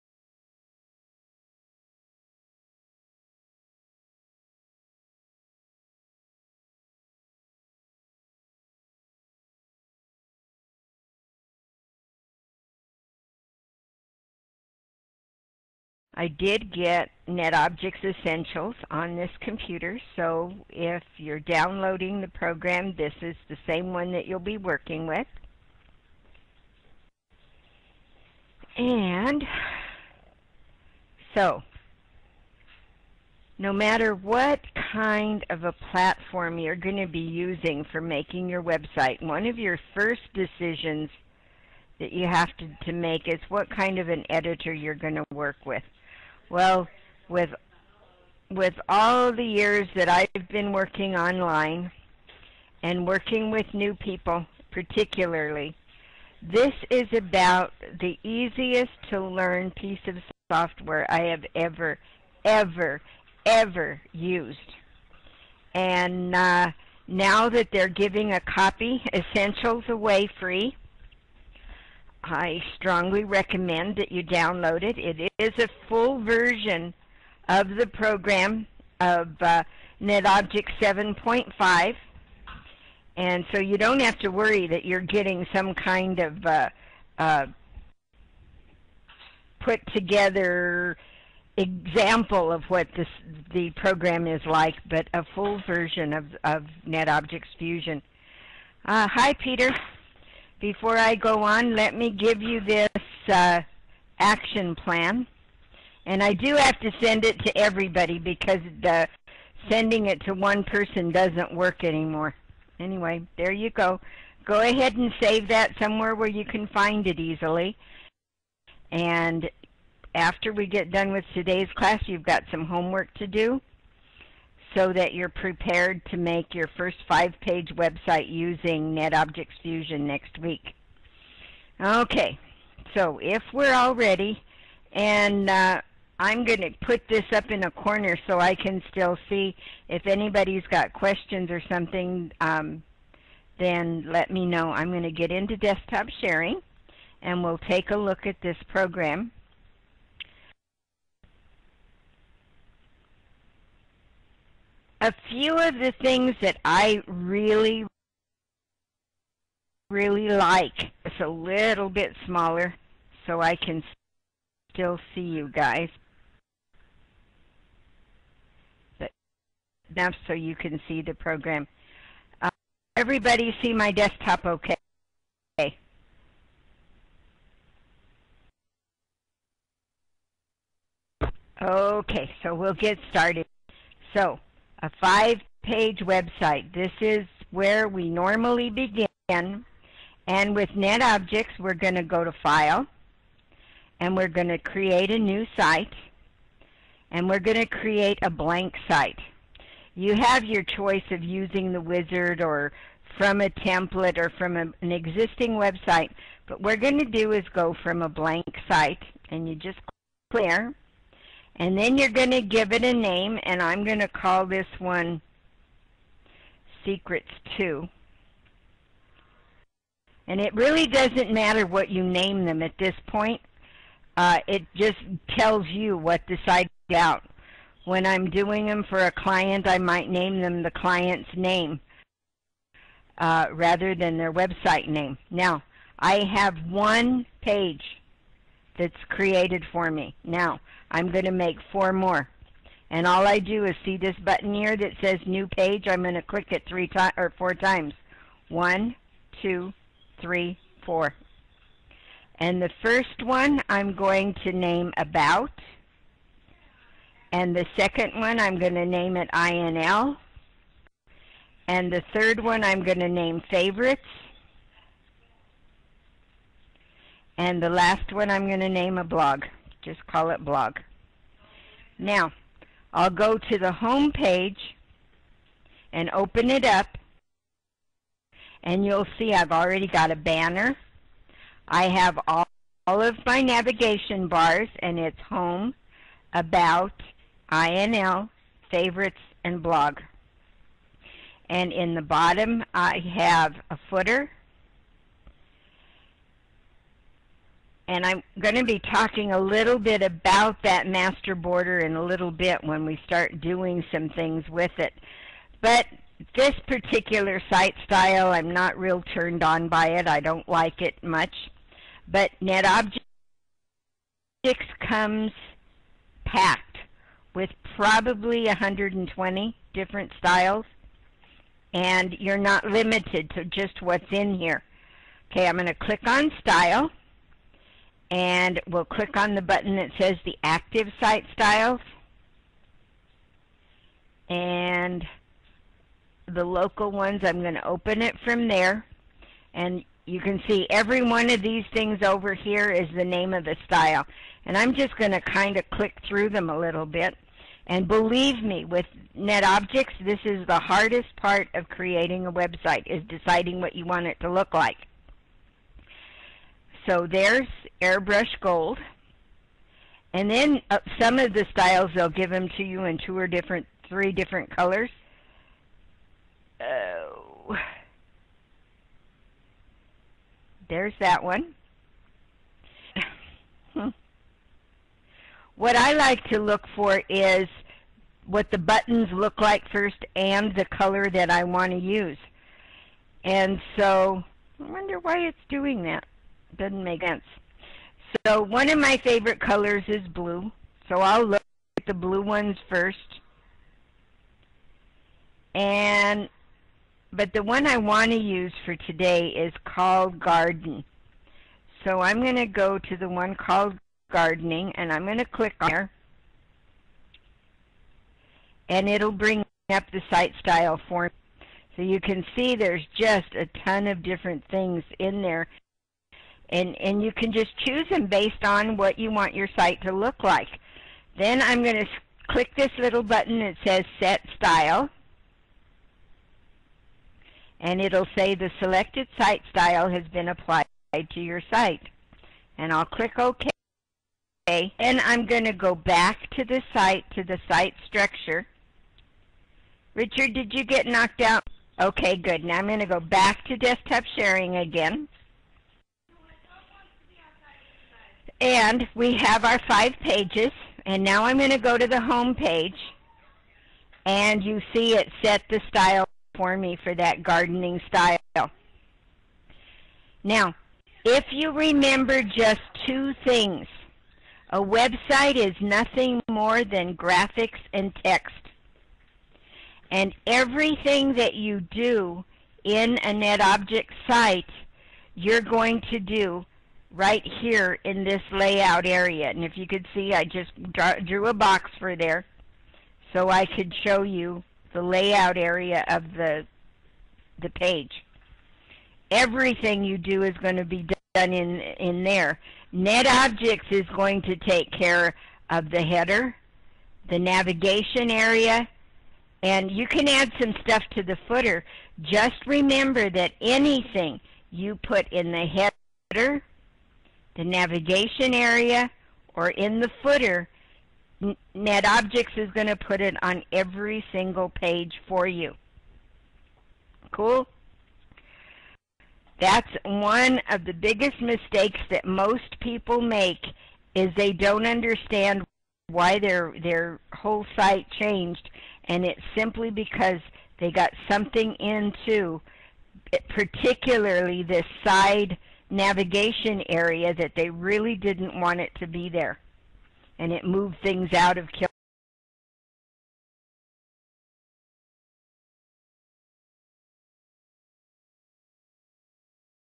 Build A 5 Page Site in Less Than A Day
Build a 5 page website in less than an hour, using the FREE program NetObjects Essesntials. This is probably the easiest WYSIWYG editor there is for a beginner or anyone that doesn't want to fuss w/ using code.